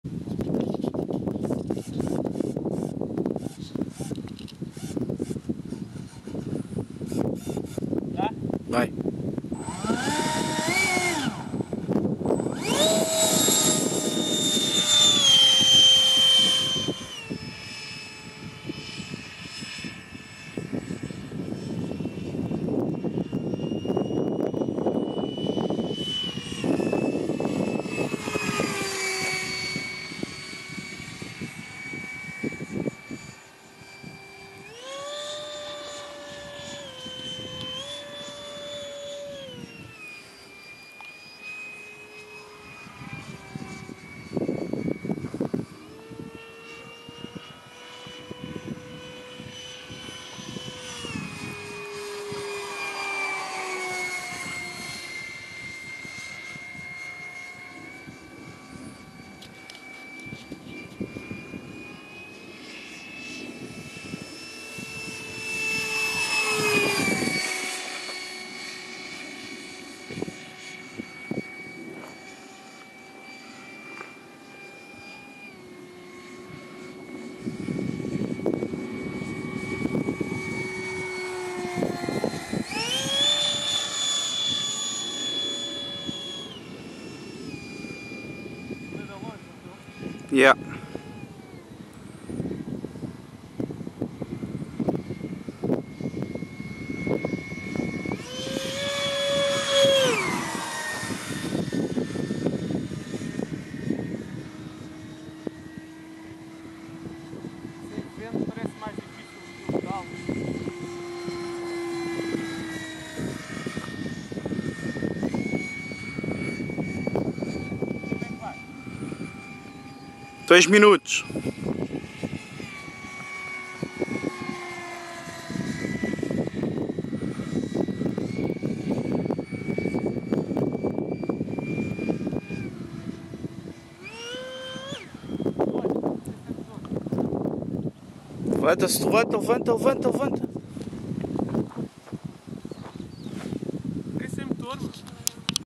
Starmax Alpha-jet 90mm, clube de aeromodelismo da Assafora, motor failure.
Aterragem de emergencia depois do motor ter parado.
o motor já era. Queimou.
Motor died.